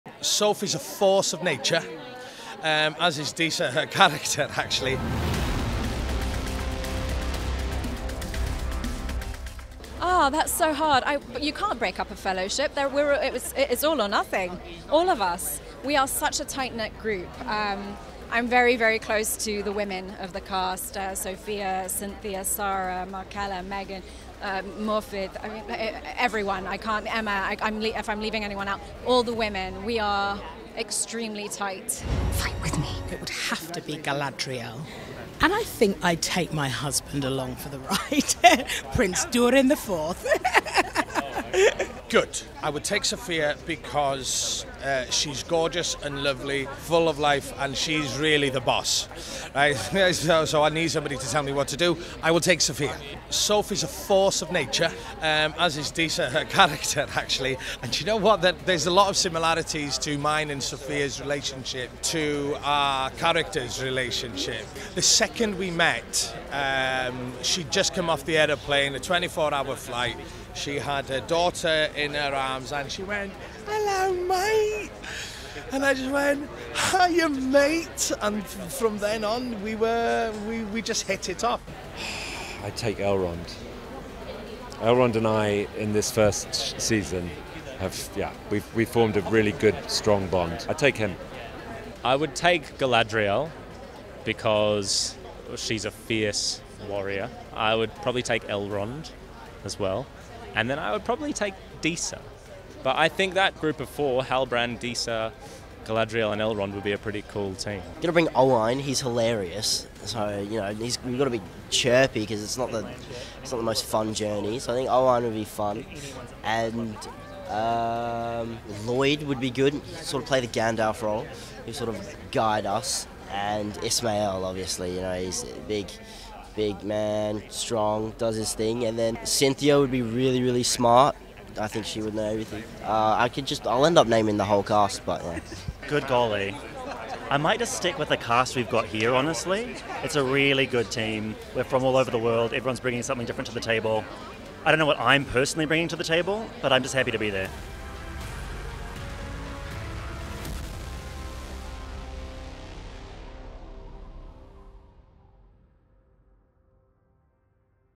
LOTR: Rings of Power Cast Answers: Who Would You Pick for Your Fellowship?
Owain Arthur (Prince Durin IV), Sophia Nomvete (Disa), Nazanin Boniadi (Bronwyn), and more cast members from The Lord of the Rings: Rings of Power make their picks for who would be in their fellowship to destroy the ring in Mordor.